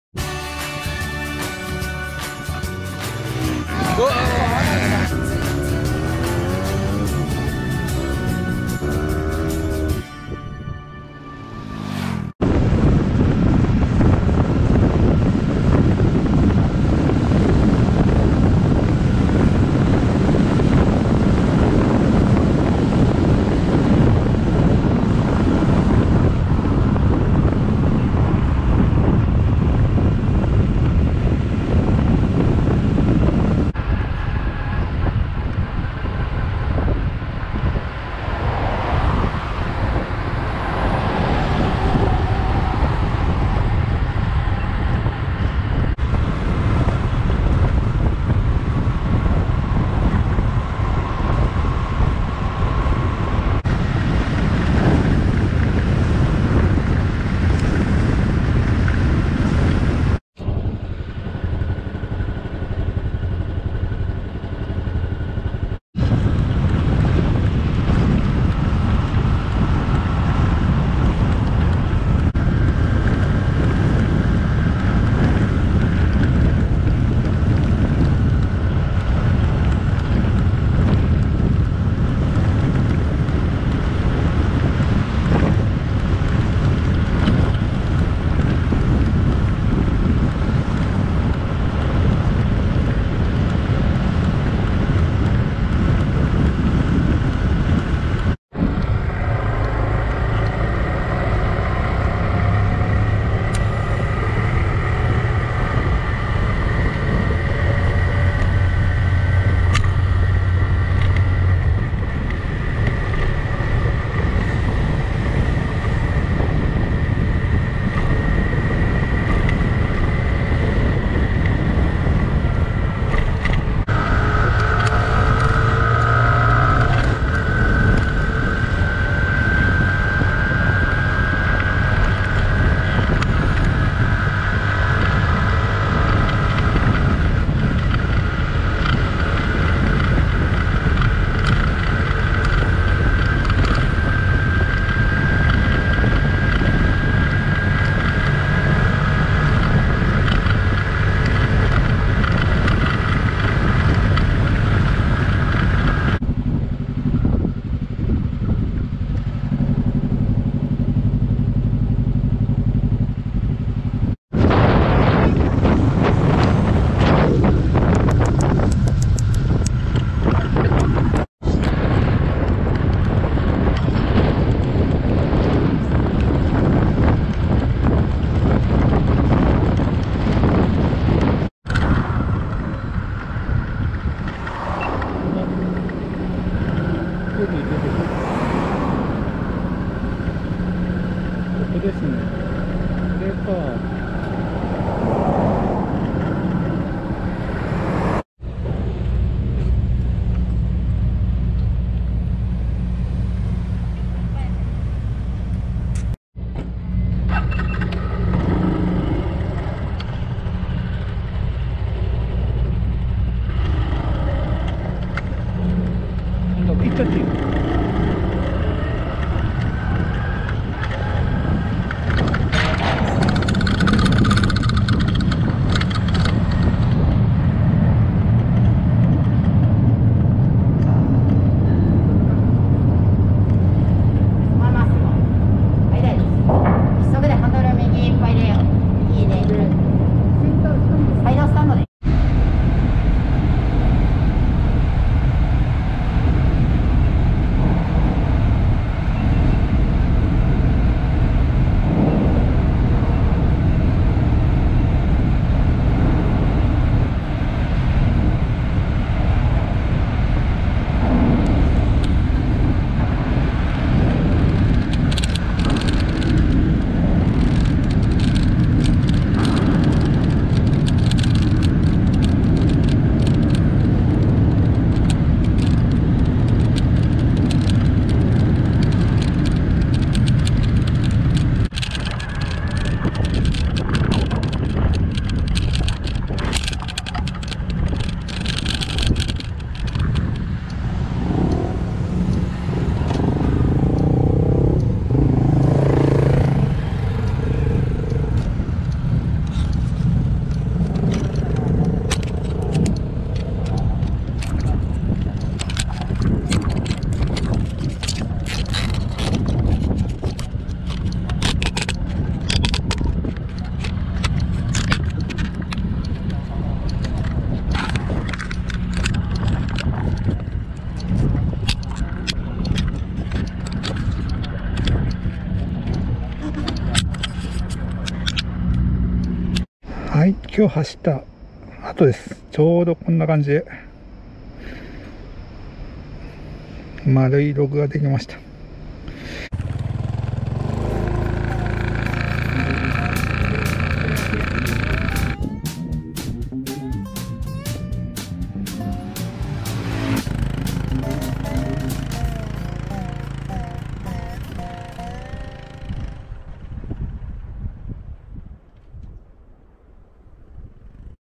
おまけ スーパーカブ無給油 国道16号1周チャレンジの時の自撮り
そのまま消去するのももったいないので
とりあえずつなげてアップしました。
どのように映るのかテストも兼ねて居たので微妙にアングルが違うと思いますＷＷ